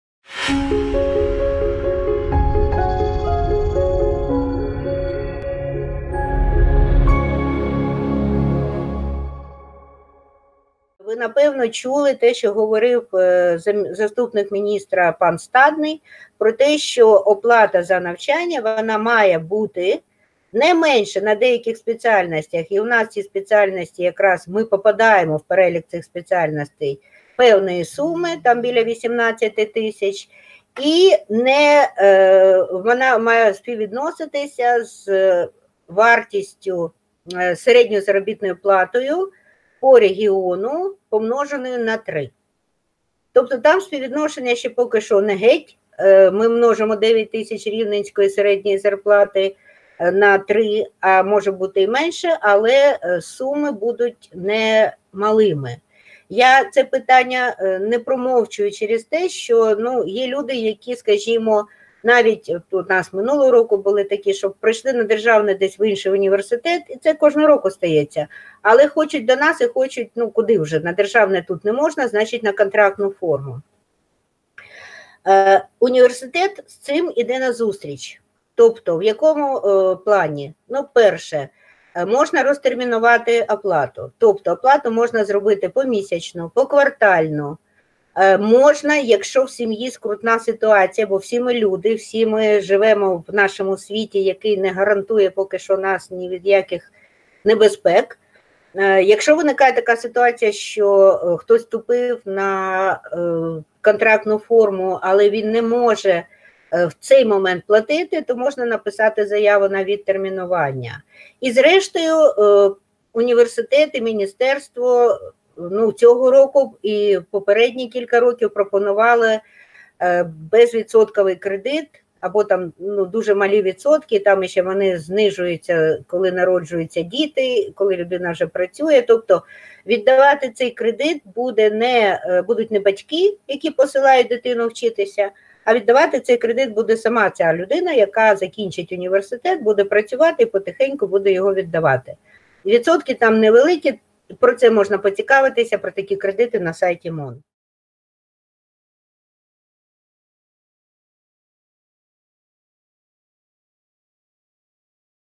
Tips36 Вступ до Острозької академії: оплата навчання та пільгові кредити
Контакти:
Приймальна комісія: (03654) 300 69,(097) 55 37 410